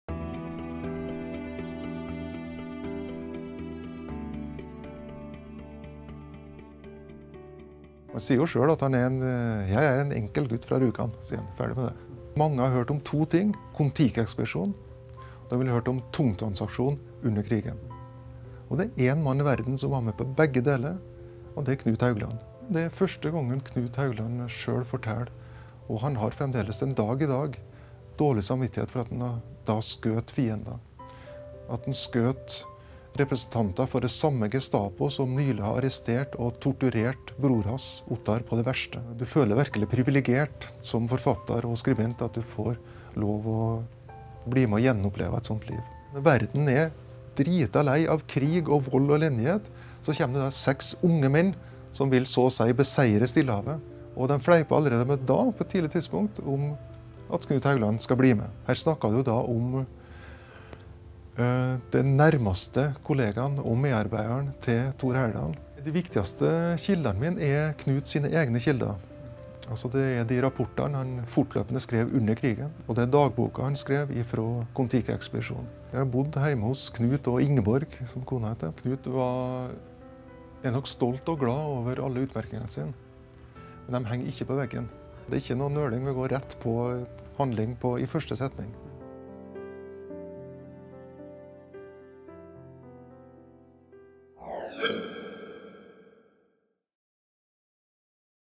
Bokfilm - Operatøren av Svein Sæter og Knut Haugland
Den 1. april 1944 skulle Knut Hauglands liv vært over. Han unnslapp døden etter å ha skutt fire Gestapo-agenter. Da hadde han allerede vært sentral i tungtvannsaksjonen, etter å ha overvintret på Hardangervidda. Senere ble han Thor Heyerdahls nære venn, deltok på Kon-Tiki-ekspedisjonen og bygde opp det berømte museet.

Knut Haugland har aldri fortalt sin historie. Før nå.